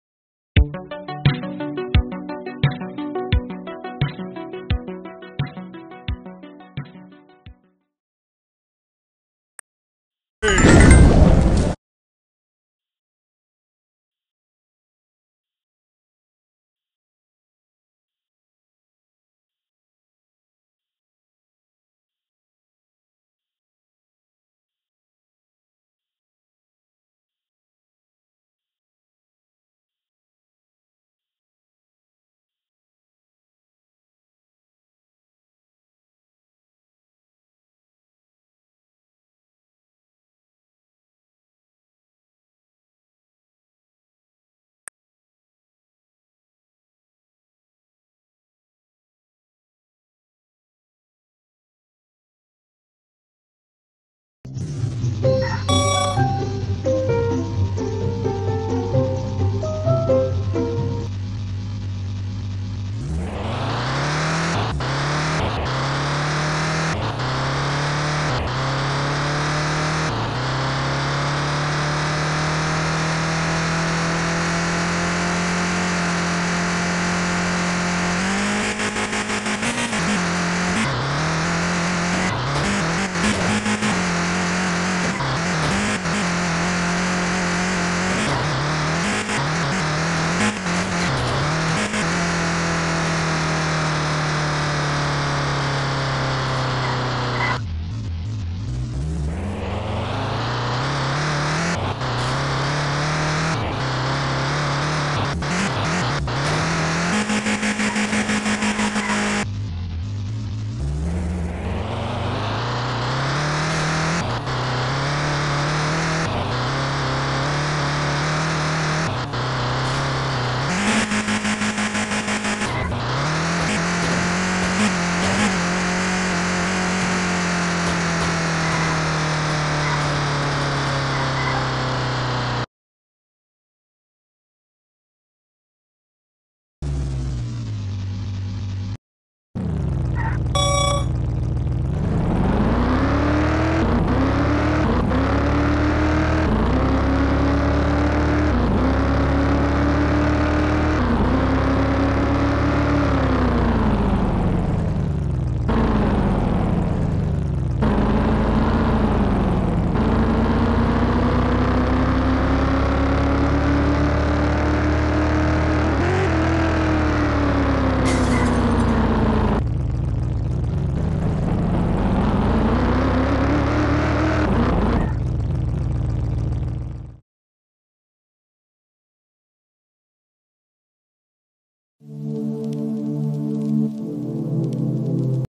iOSs beam.ng clone that’s kinda fun
This here is a beam.ng clone on the apple App Store that’s not actually that bad